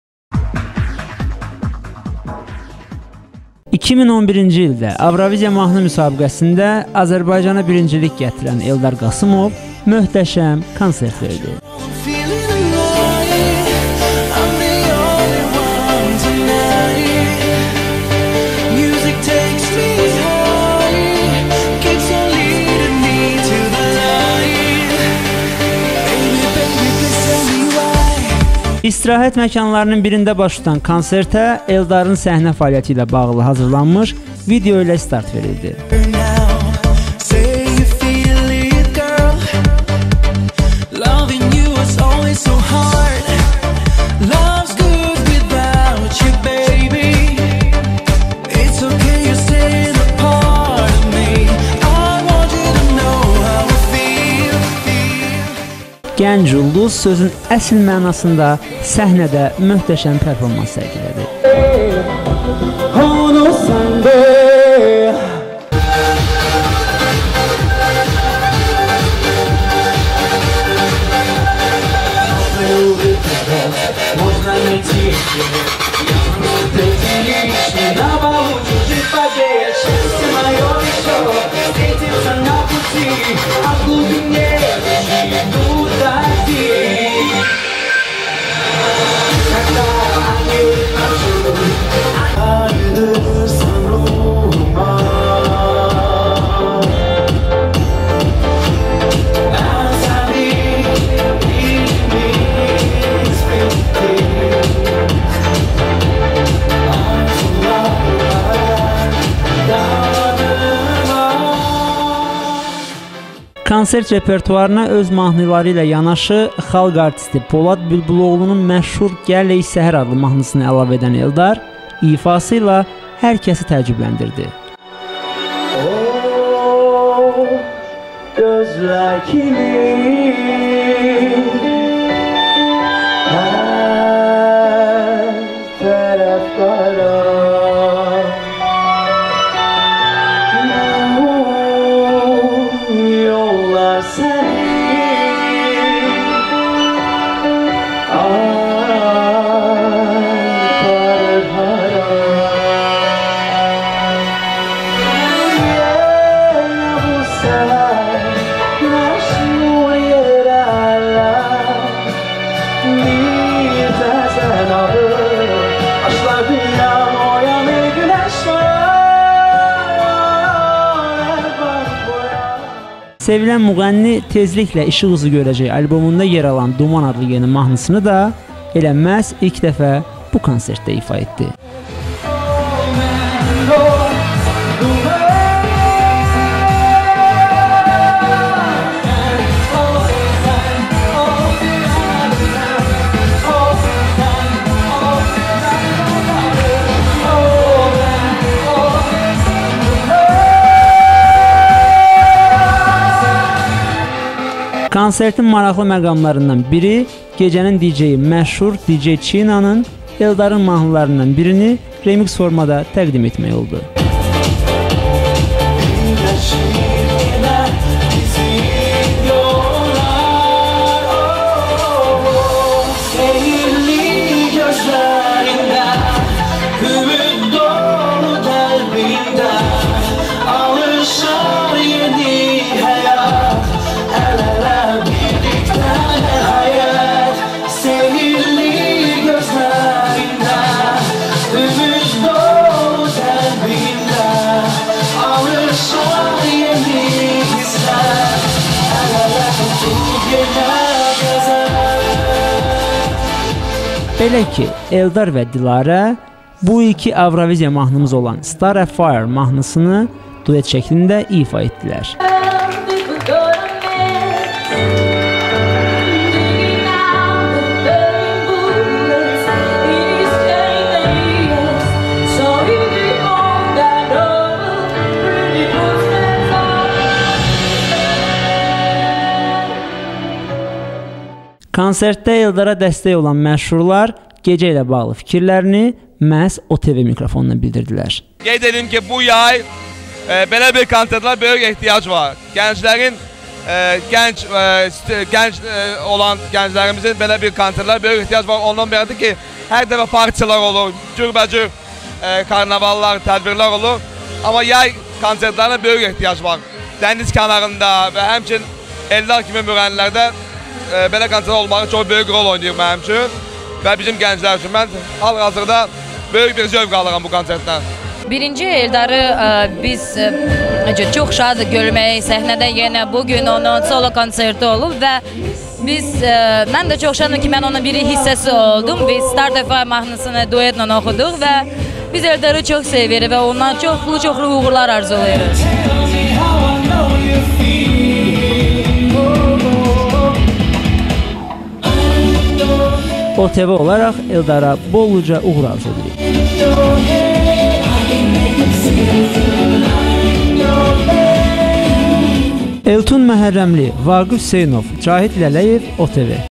Eldar Qasımovun solo konserti
Azərbaycanı 2011-ci ildə Avroviziya mahnı müsabiqəsində birincilik gətirən Eldar Qasımov möhtəşəm konsert verdi. Daha ətraflı video süjetdə.